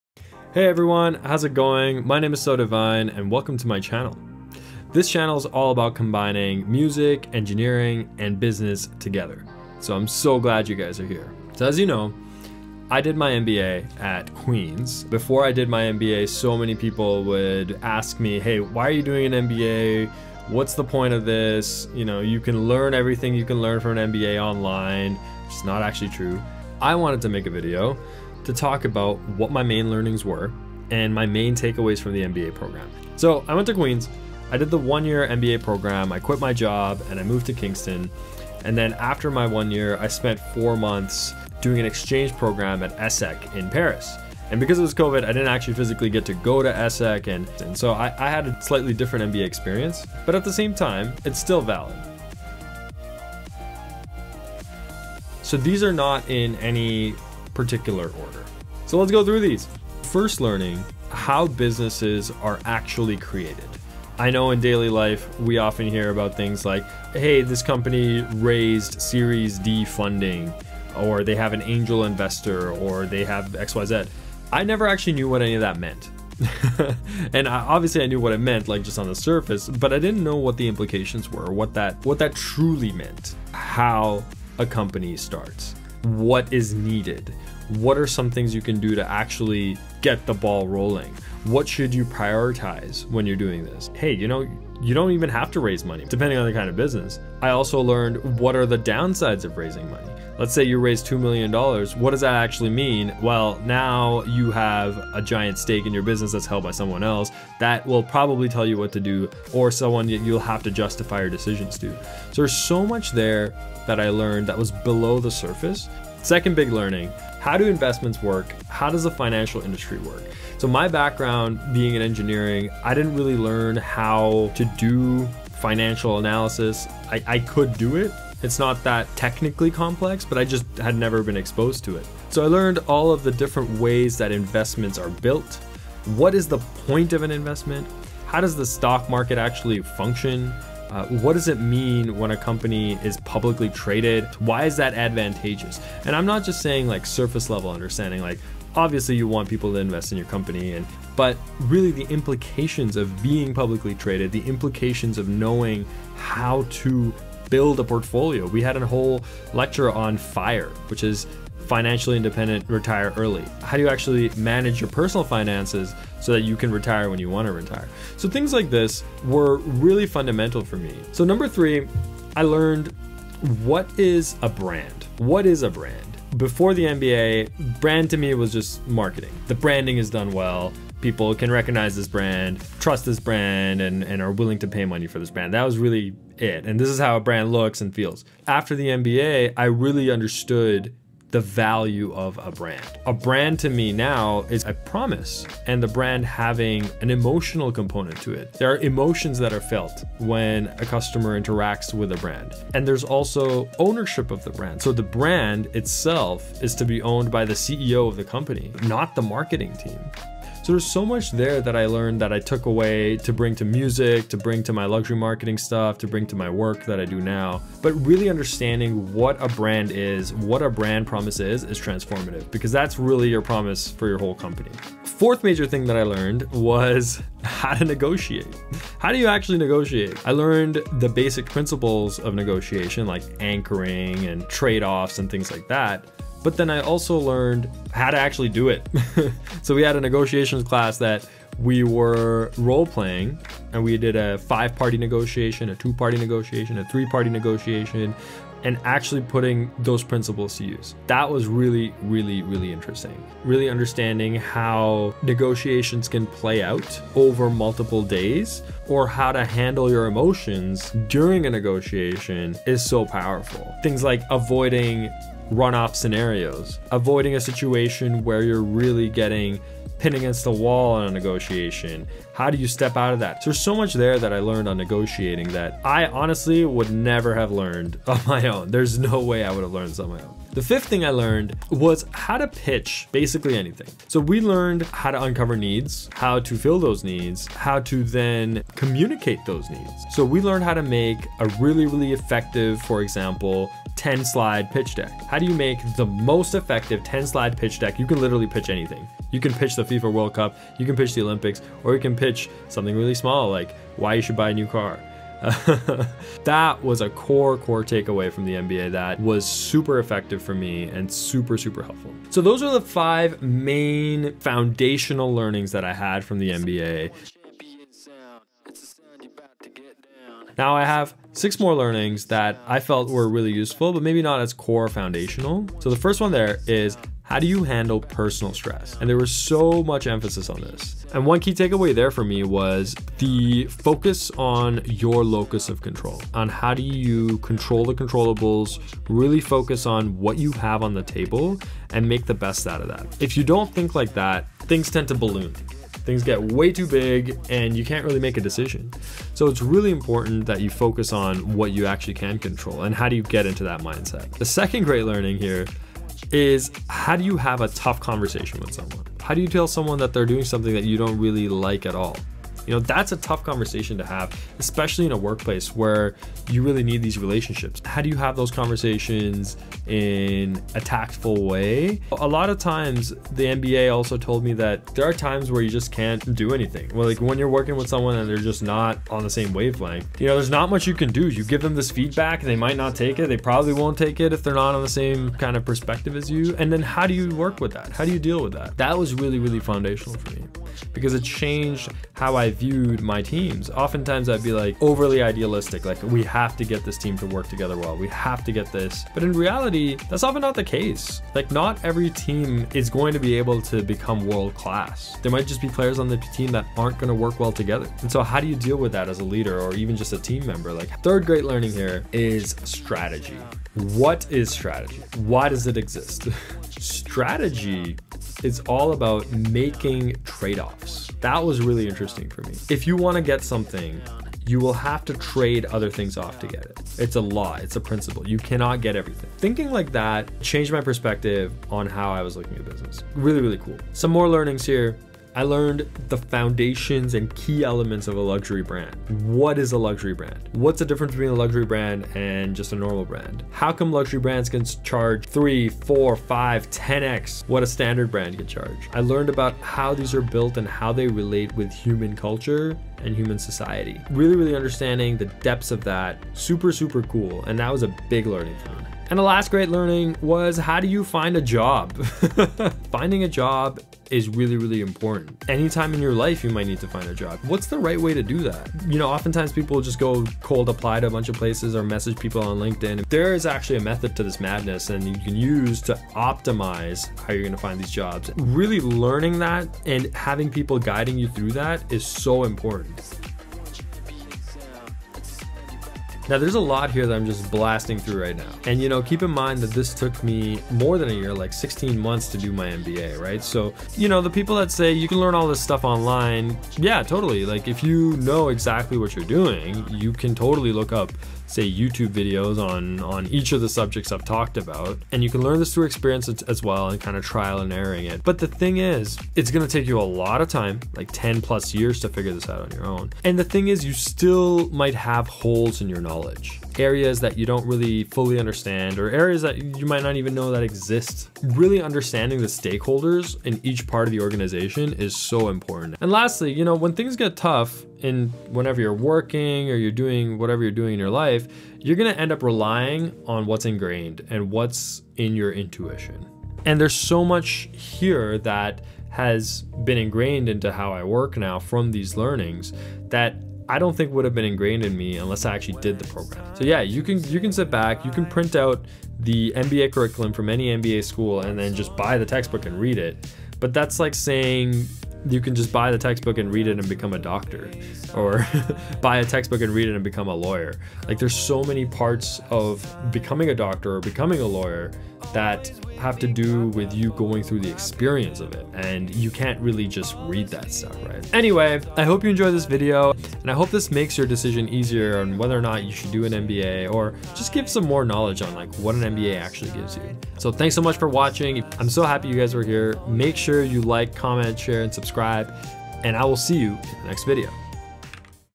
Core learnings from my MBA | Sodhivine
Here is some insight into what I learned from my MBA program, I hope this is useful for you!